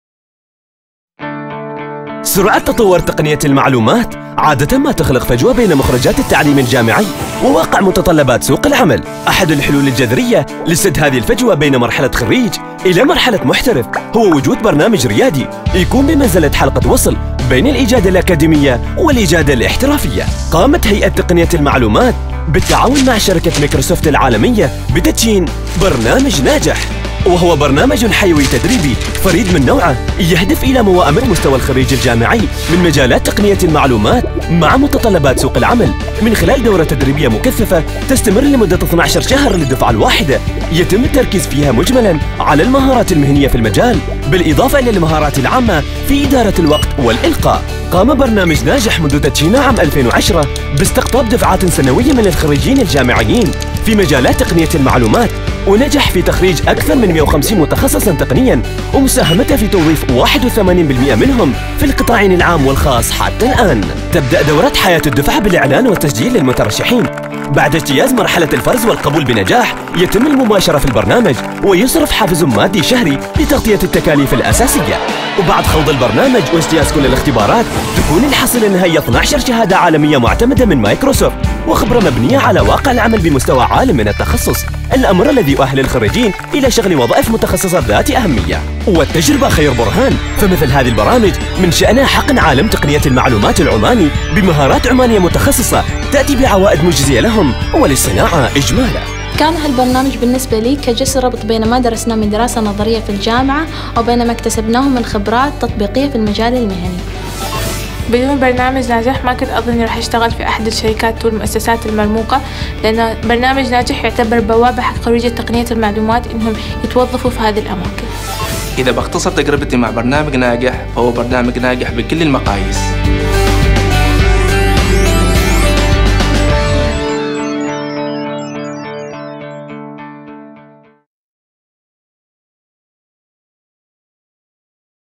Najeh Program
Najeh is a one year training program provided by ITA for fresh IT graduates in cooperation with Microsoft - ناجح هو برنامج تدريبي لمدة عام تقدمه هيئة تقنية المعلومات  لخريجيي تقنية المعلومات بالتعاون مع شركة مايكروسوفت